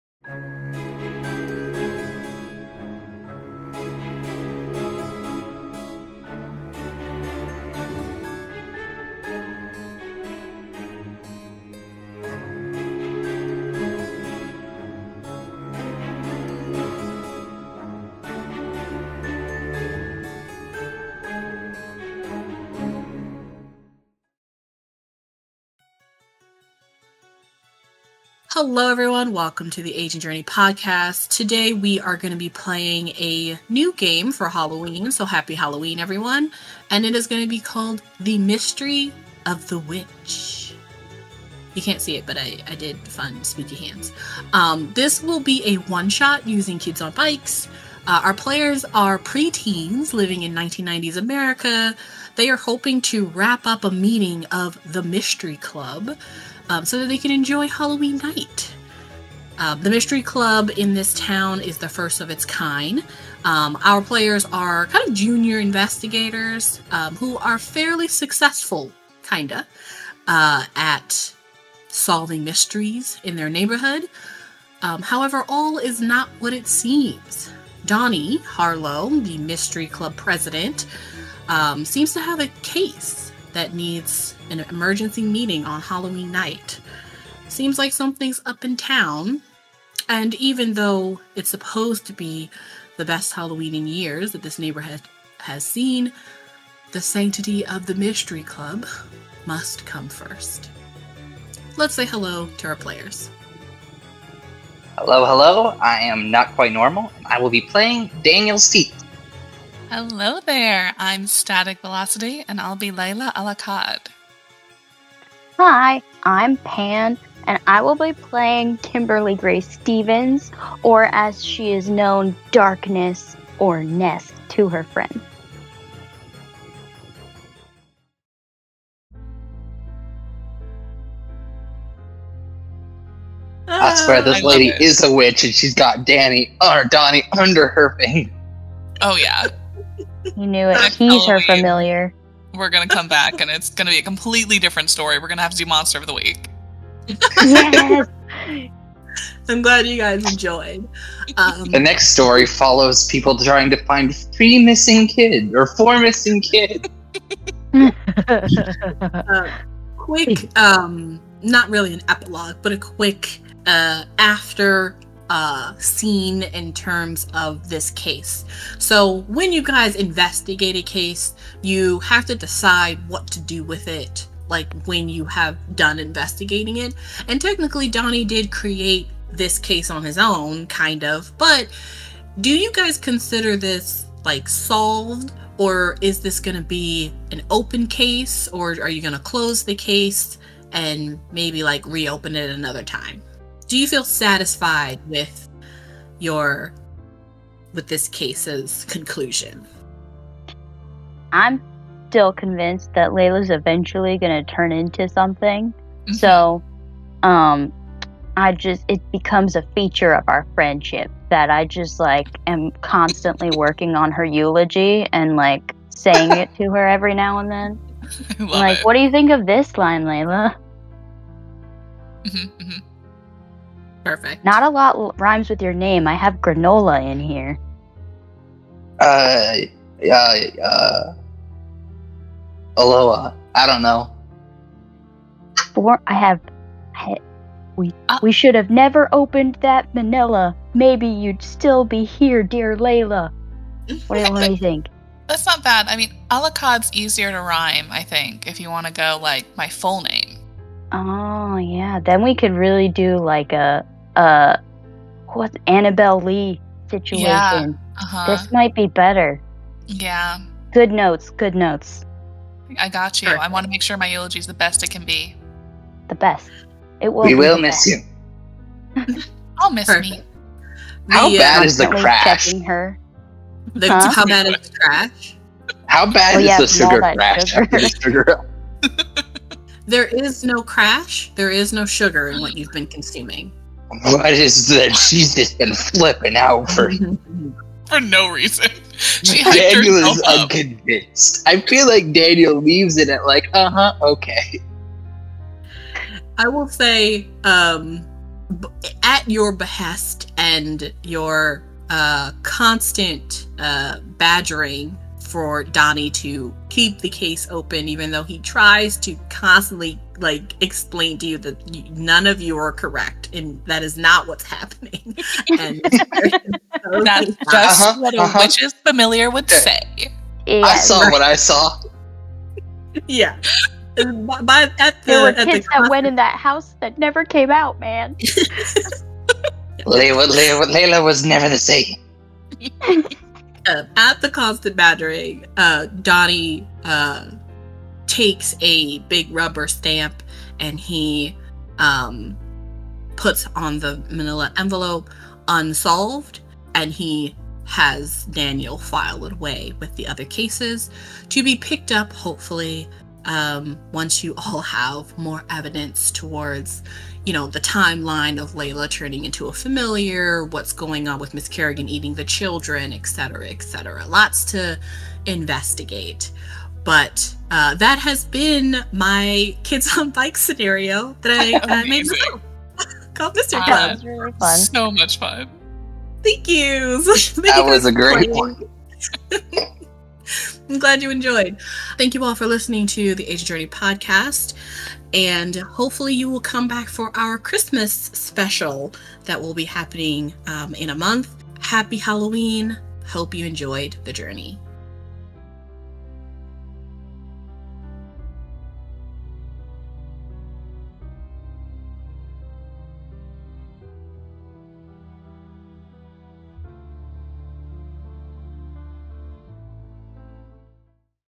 The Mystery of The Witch / Mystery Club: A Kids on Bikes Halloween Scenario / Epilogue
Let's go back in time to the heyday of Halloween, the 90s, with this original scenario written by GM Tanya.

It's Halloween night. Children in the neighborhood are saying this will be the best year for candy, almost every house will be hosting boozy Halloween parties, and Ms. Carrigan has had a big sign on her lawn all September that reads "Best Haunted House in the Neighborhood". This year's Halloween is going to be epic, but you won't be enjoying any of the fun until after the meeting. Donny Harlow, president of the Mystery Club has lost it! Calling an emergency meeting on Halloween night. You and your friends tried to get out of it but no dice. Anyone who misses this meeting will be kicked out of the Mystery Club.

V.P. Pan as Darkness aka Ness
Administrator Notquietnormal as Daniel
Investigator Static as Layla